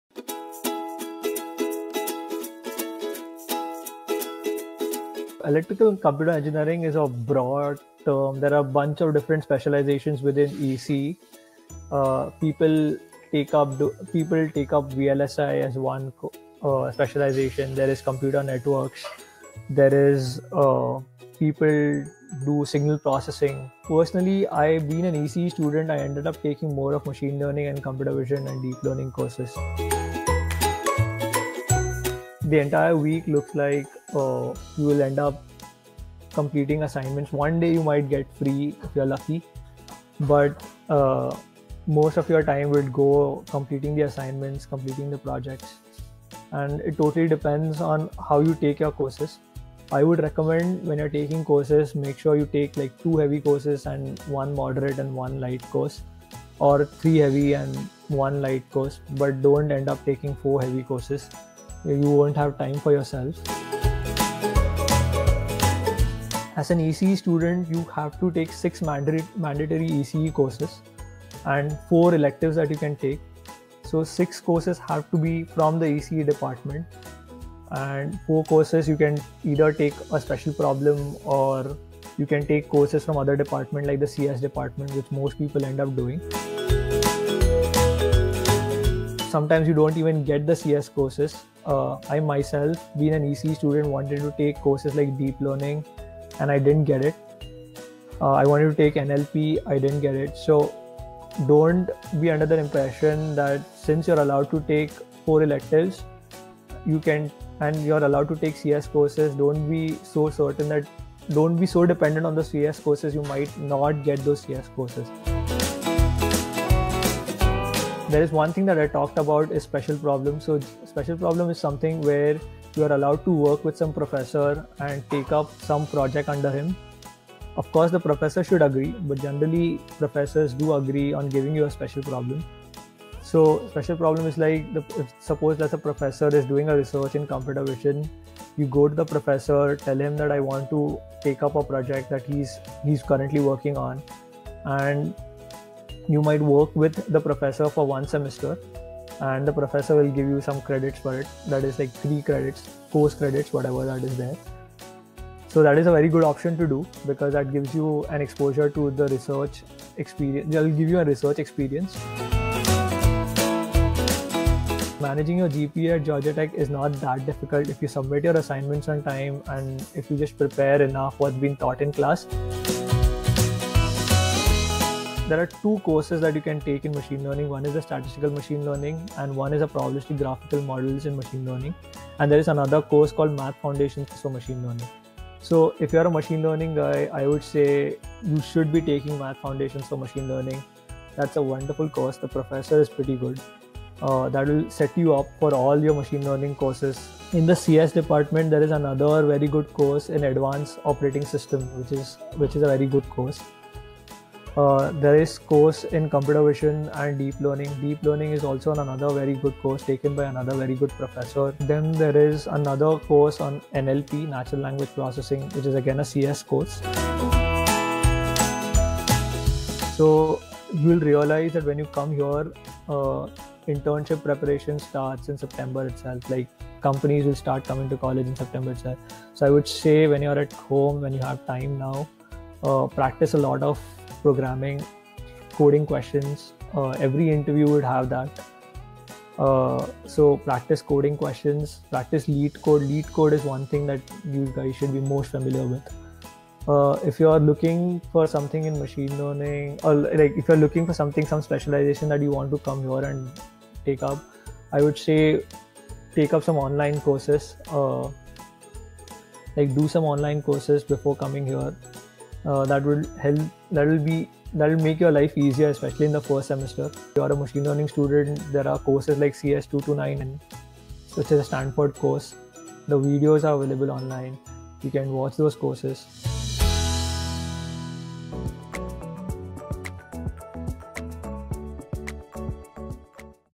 Electrical and Computer Engineering Program at Georgia Institute of Technology
Meet Rahul, a Collegepond Alumnus and graduate of Georgia Institute of Technology. He shares with you crucial firsthand information about academics at GIT... right from insight into the course, class diversity and profile, credit requirement, coping with academic pressure, and more